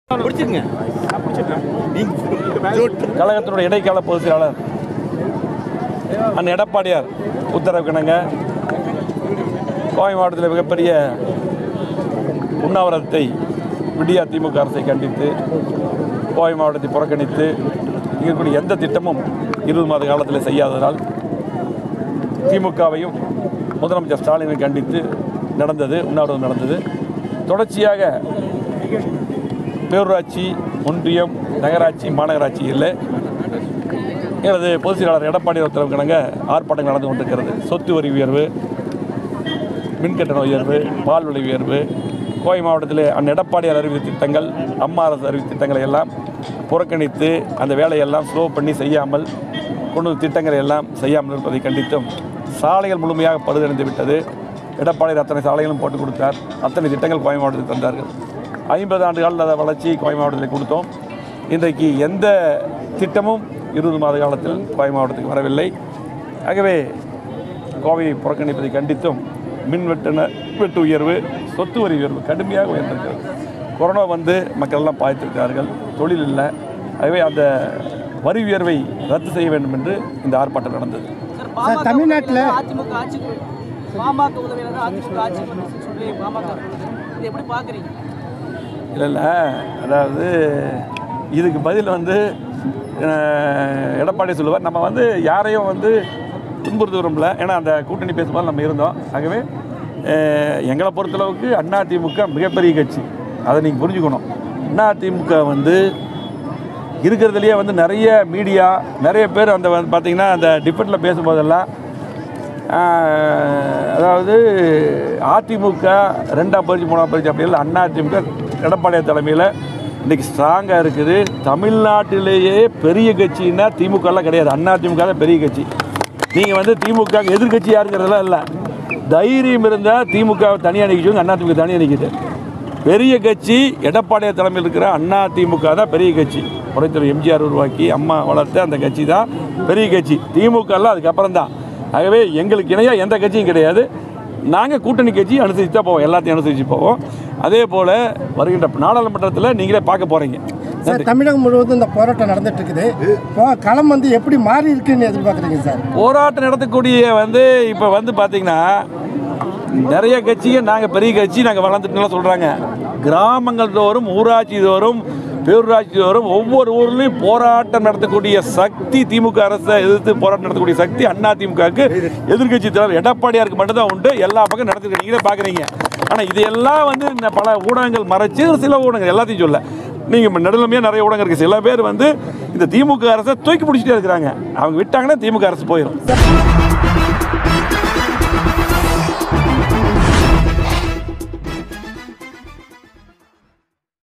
”தமிழ்நாட்டில் அதிமுகவிற்கு இணையான வேறு கட்சி இல்லை” - எஸ்.பி. வேலுமணி பேட்டி பாமகவுக்கு பதிலடி
பாமக வழக்கறிஞர் பாலுவின் கருத்து கேள்விக்கு, ”இதற்கு எடப்பாடி பழனிசாமி பதில் சொல்வார்” என பதிலளித்தார். தொடர்ந்து பேசிய அவர், “ யாரையும் புண்படுத்த விரும்பவில்லை. கூட்டணி பேசும் போது நானும் இருந்தேன். எடப்பாடி பழனிசாமி தலைமையிலான அதிமுக தான் தமிழ்நாட்டில் மிகப்பெரிய கட்சி. தமிழ்நாட்டில் பெரிய கட்சி திமுக கிடையாது. அதிமுக தான். எடப்பாடி பழனிசாமி தலைமையிலான அதிமுக பலமாக உள்ளது.